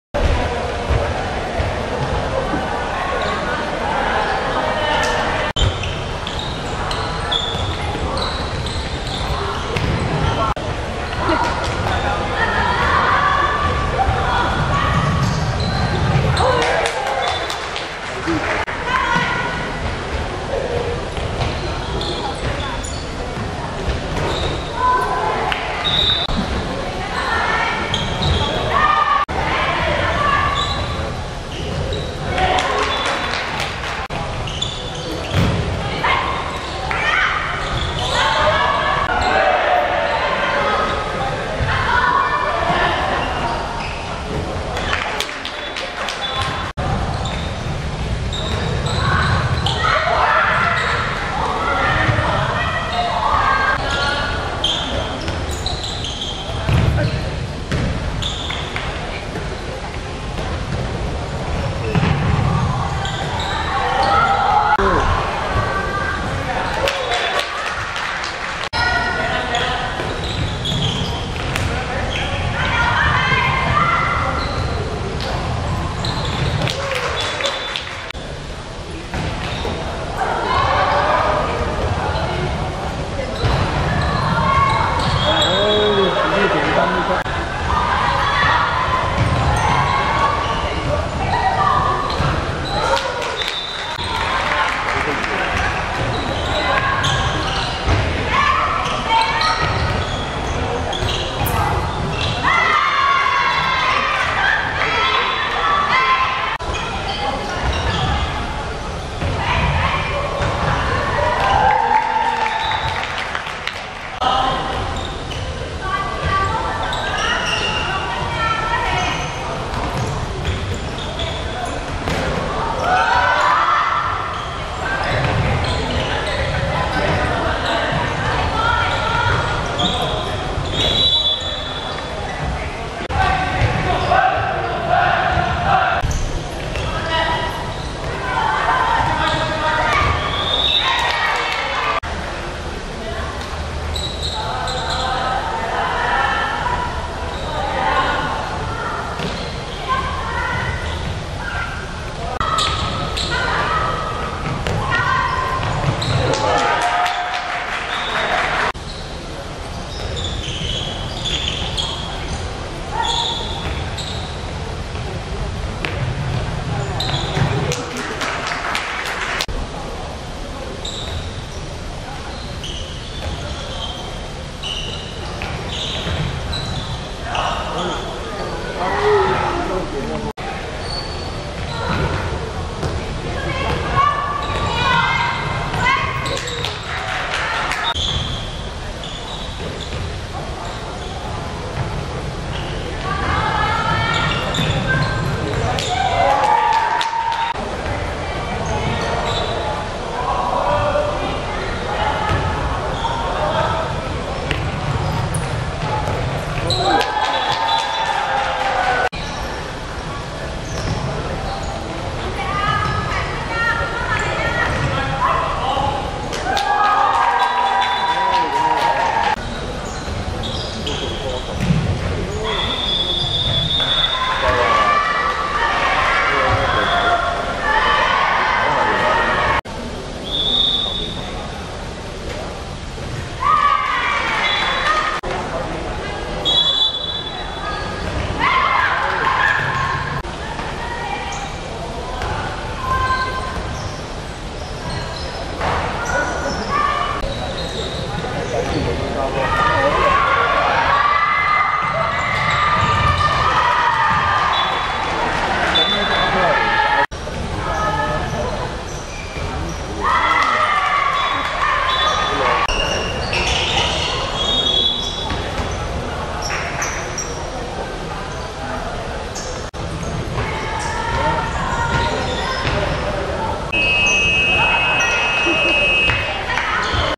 鄉議局vs協恩(2016.1.31.學界女子手球精英賽八強)精華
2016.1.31.中山紀念公園體育館 11:15 鄉議局(白) 5 : 14 協恩(藍)